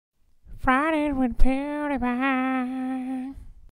PewdsIntro | Fridays with PewDiePie intro | First intro I've made
This is the first intro I ever have made.
Please tweet this and PewdsIntro @PewDiePie so he can see!